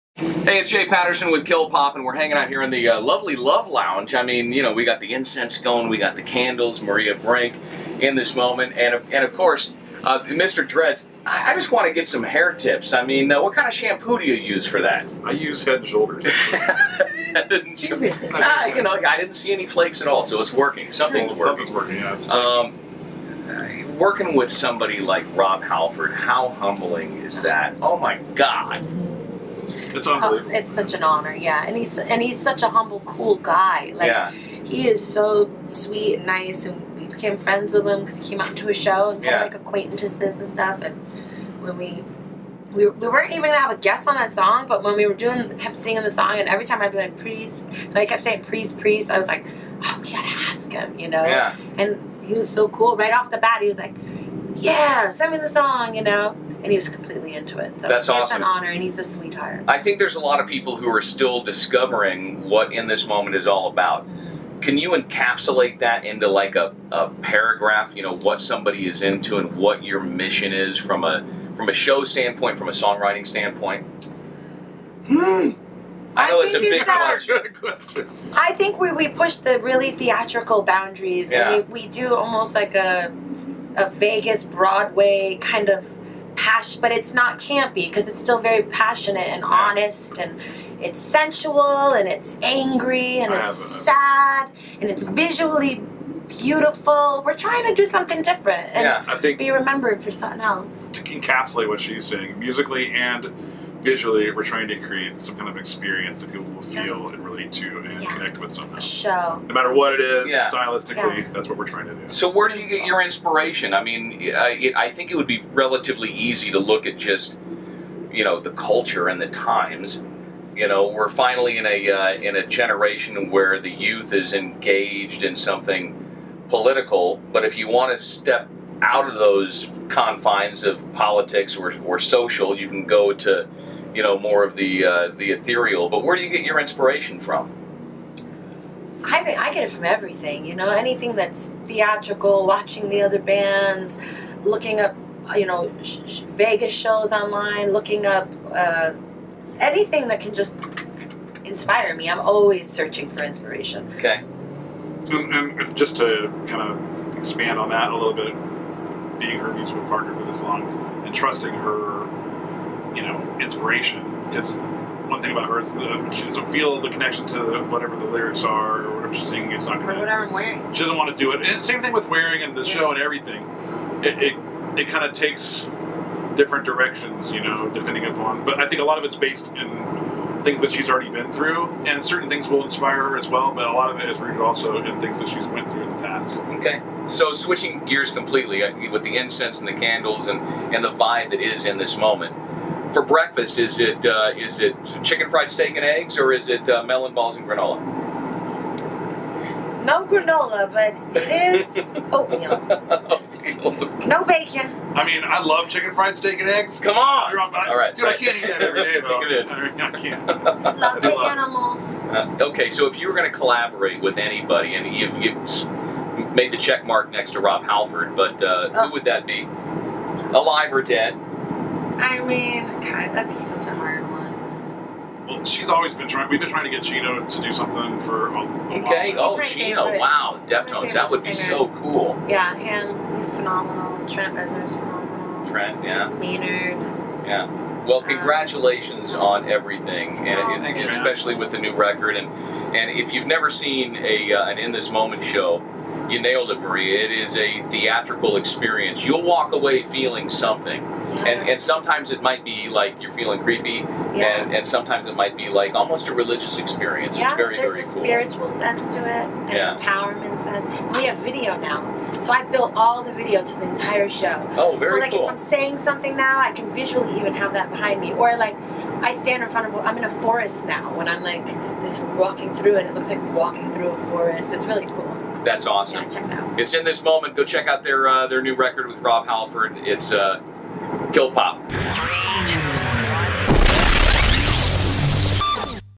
In This Moment at Las Rageous Kilpop Stage Left
Kilpop Stage Left: Have Patterson interviews In This Moment at Las Rageous in Las Vegas, Nevada.